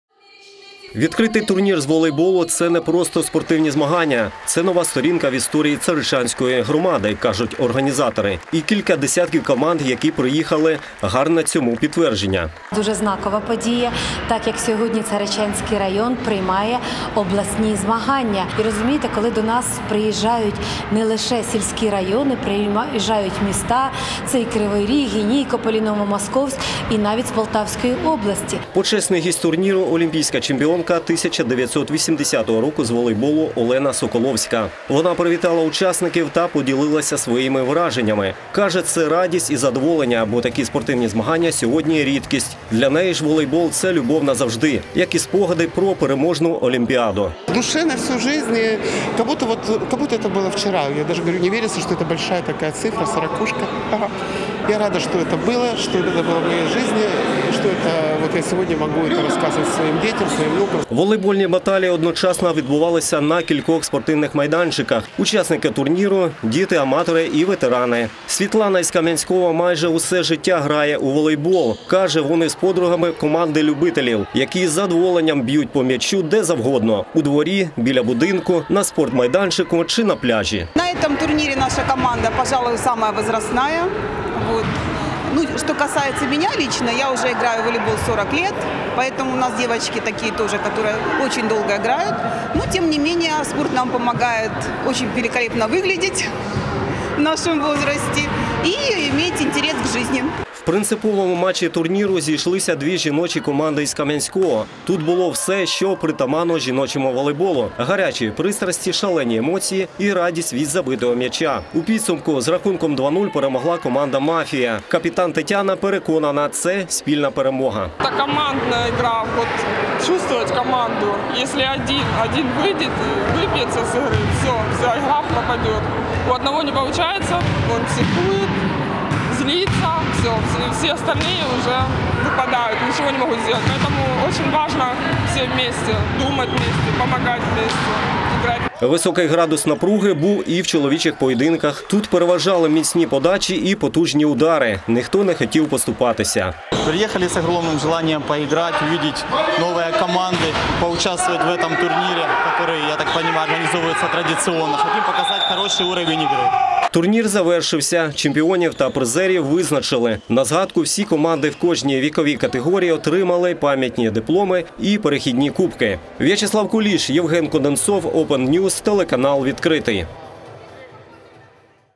У Царичанській громаді відбувся турнір з волейболу
Спортивне свято у Царичанській громаді. Там пройшов відкритий обласний турнір з волейболу «Маю честь запросити». Його присвятили 40-річчю тріумфу збірної колишнього Радянського Союзу на олімпійських іграх 1980 року.